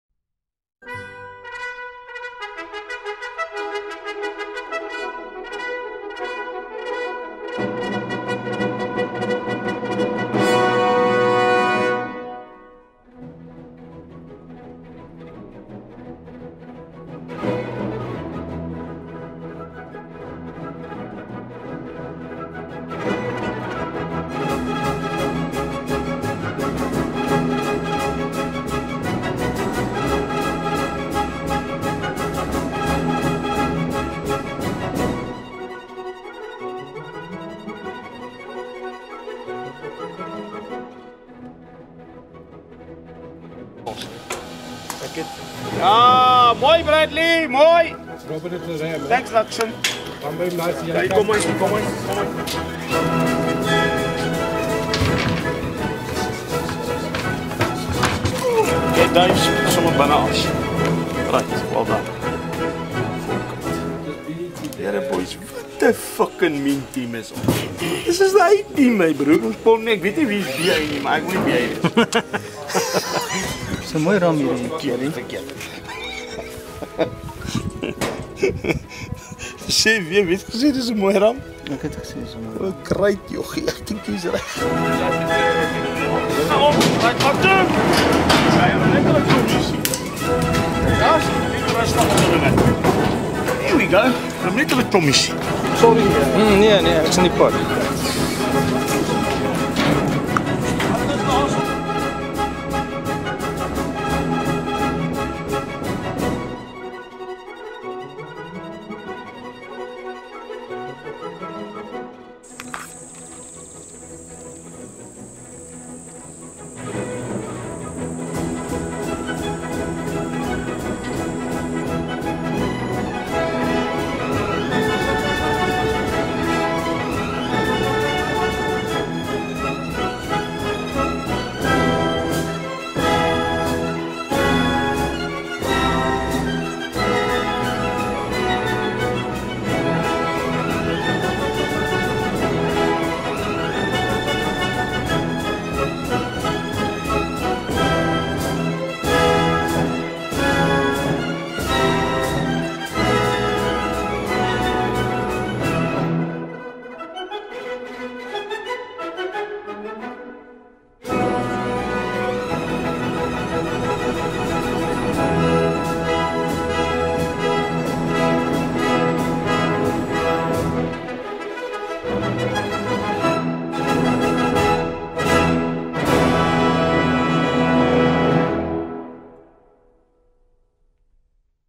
Kromdraai Wildvangs
Moving animals from Kromdraai in Limpopo to Koedoeskroon near Sun City.